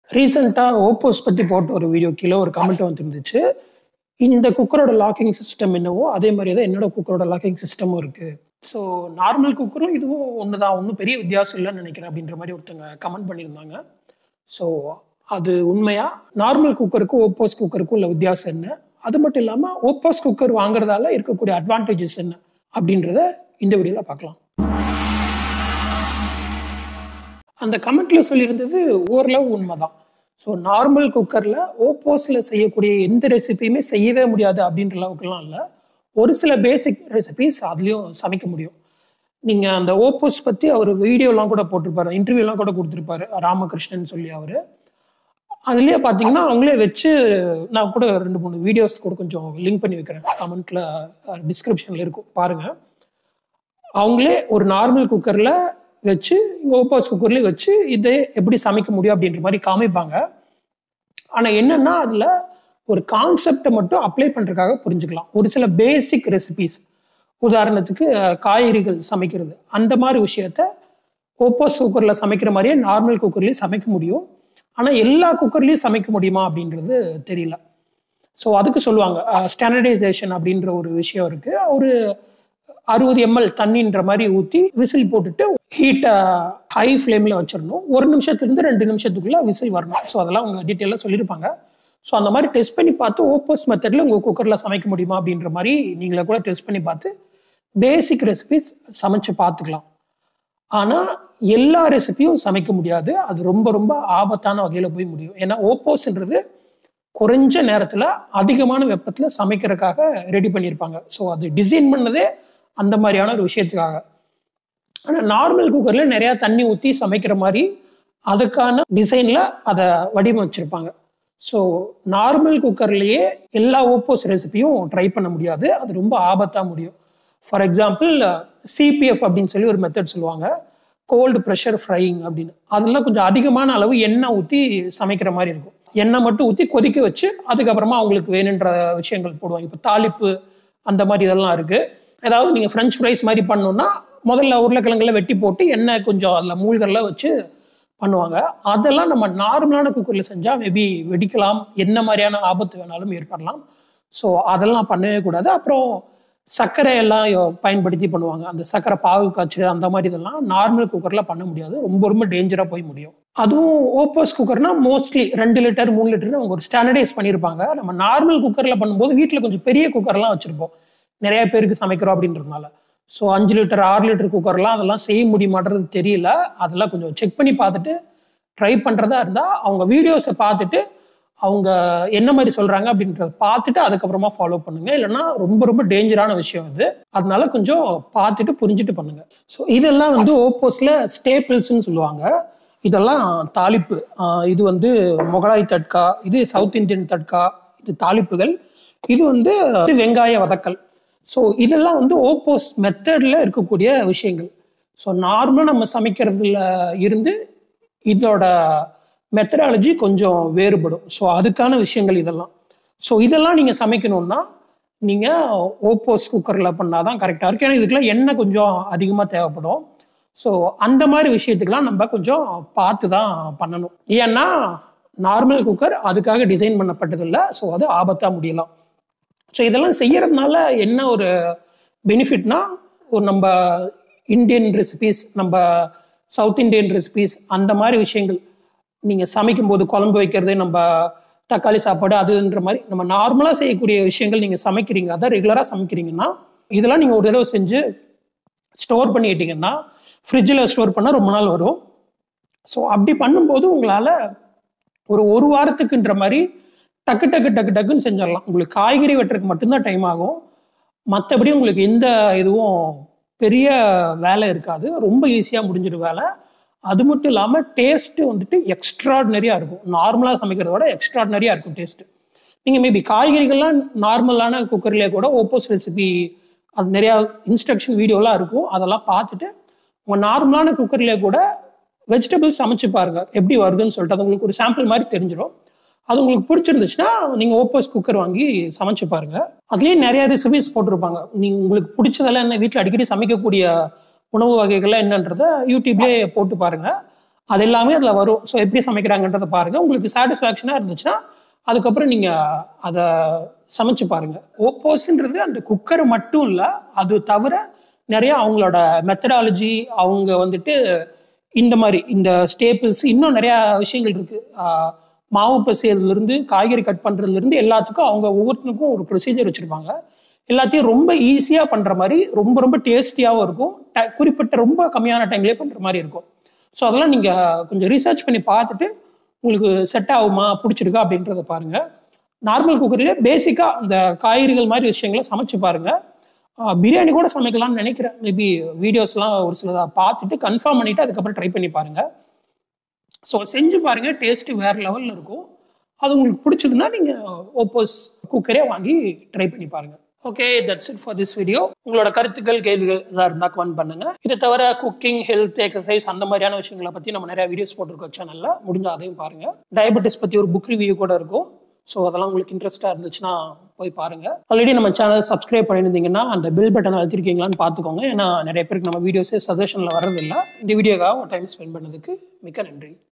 OPOS Cooker vs Normal Cooker; What's the Difference? ஓபோஸ் குக்கர் | Ramakrishnan | Pressure Baker
My Carnivore Diet Experience  Tamil

சளியில்லா உணவுமுறையில் எனது 5மாத அனுபவம்

Comment your thoughts to make this channel meaningful :)

LIVE, LOVE AND PEACE!